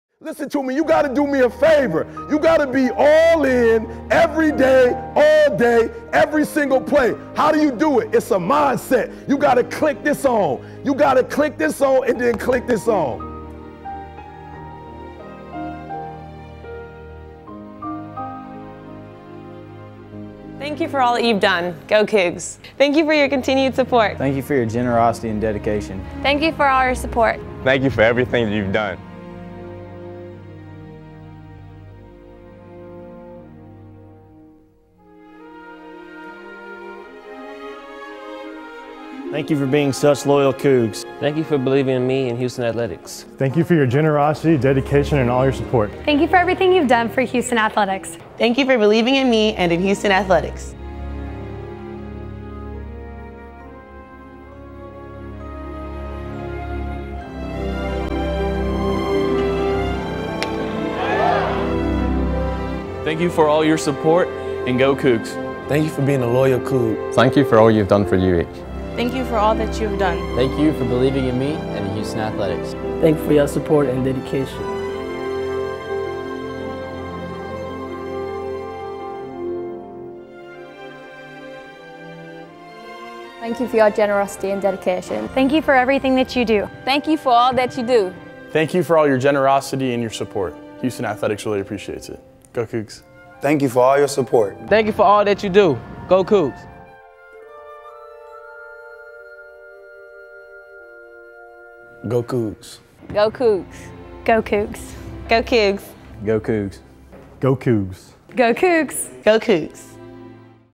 Corbin J. Robertson Dinner : Thank You Video
Check out how Cougar student athletes say thank you to their donors.